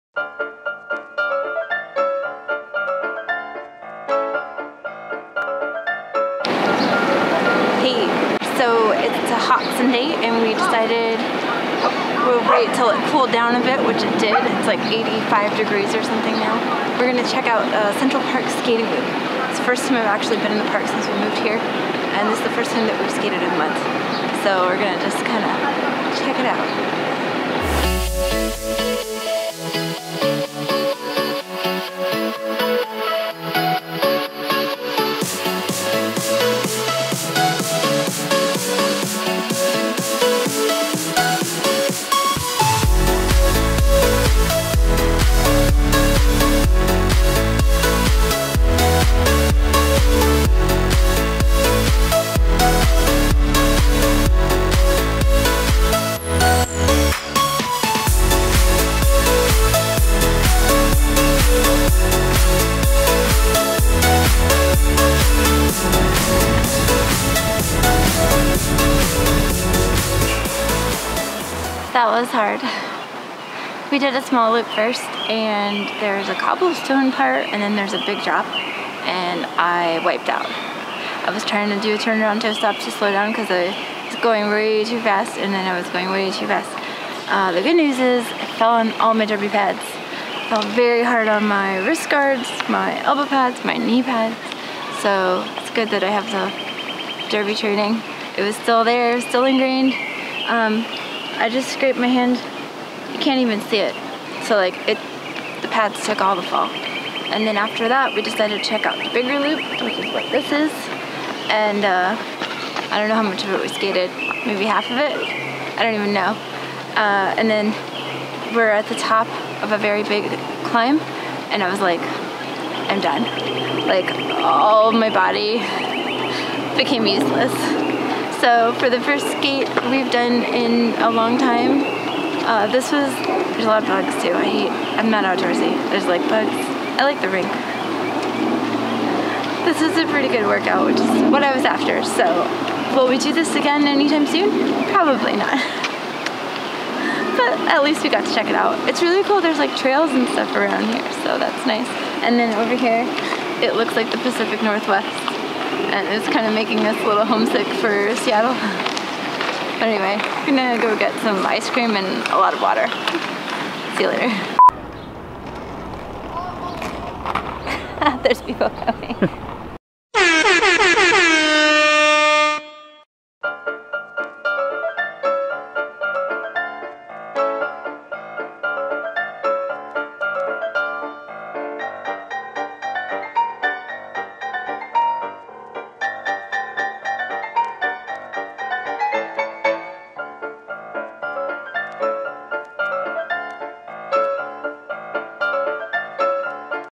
Skating The Loop in Central Park! NYC / New York City Rollerskating
We decided to go check out different parts of Central Park's Loop on skates on a very hot Sunday in June!

New York City, Rollerskating, trail skating, Rollerskating in NYC